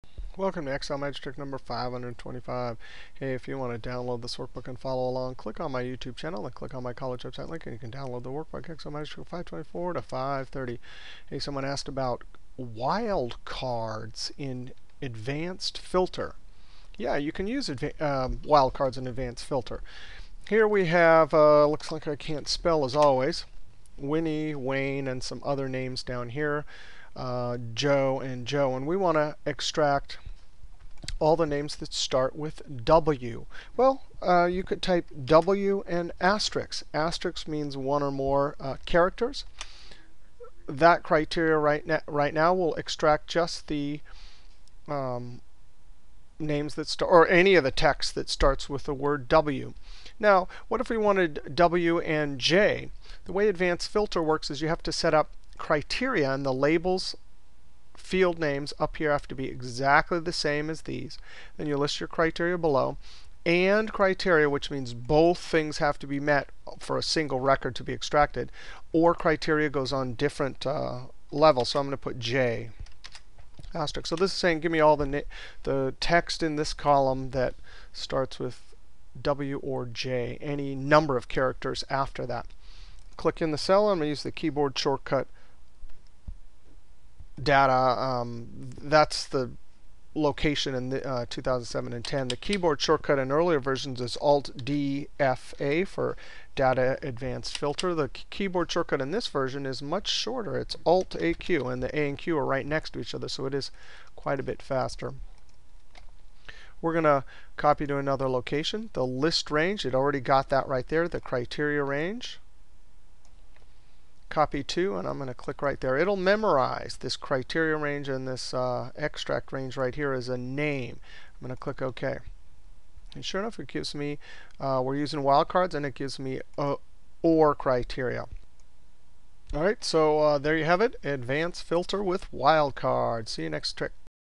Excel Magic Trick 525: Advanced Filter With Wild Cards
See how to extract records using Advanced Filter and criteria that uses Wild Cards. See how to extract records that start with the letter W or J.